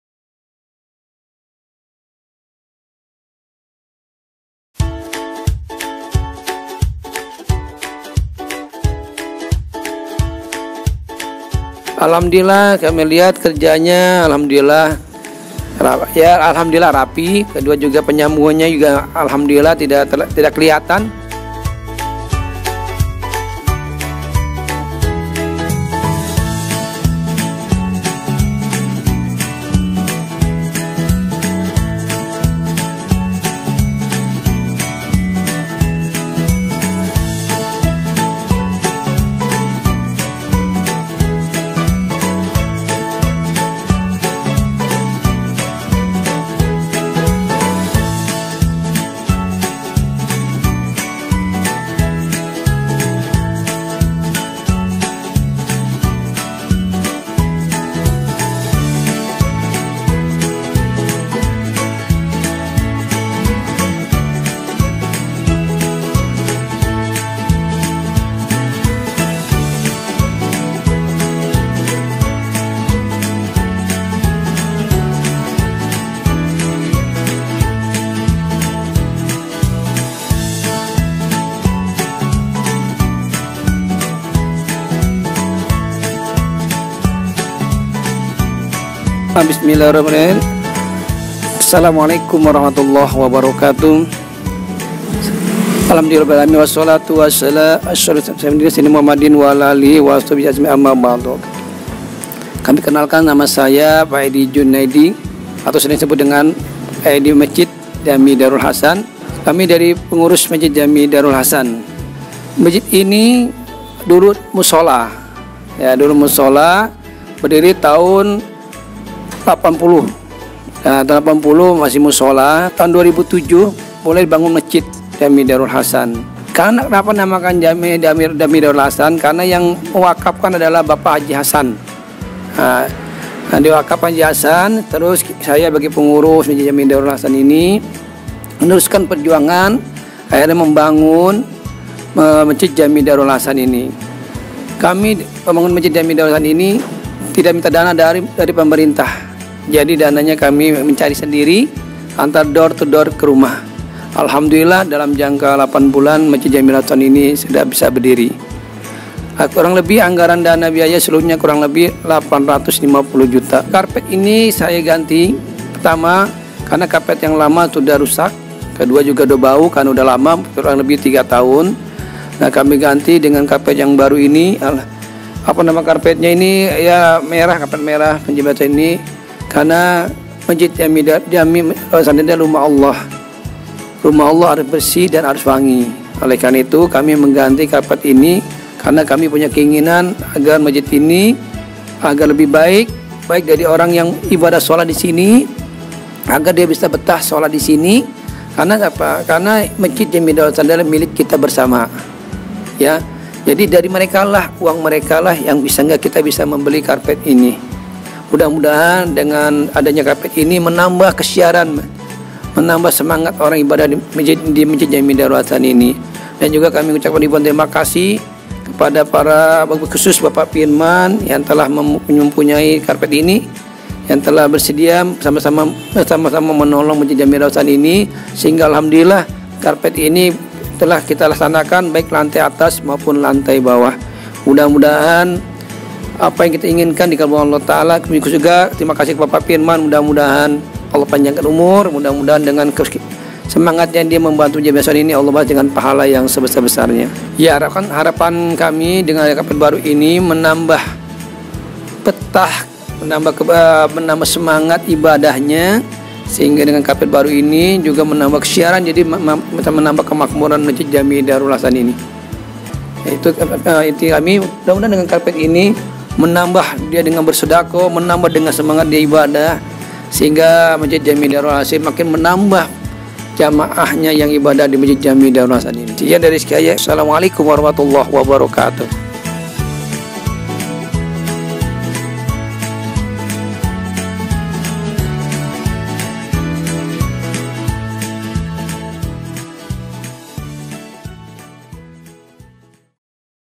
PEMASANGAN KARPET MASJID JAMI DAARUL HASAN, PEJATEN BARAT-JAKARTA SELATAN
Alhamdulillah tim dari Sahabat Karpet telah selesai melaksanakan pemasangan karpet di Masjid Jami Daarul Hasan, Pejaten Barat-Jakarta Selatan, semoga dengan adanya karpet baru ini, akan menambah kenyamanan dan kekhusyukan para jamaah masjid untuk beribadah kepada Allah SWT.. Aammin..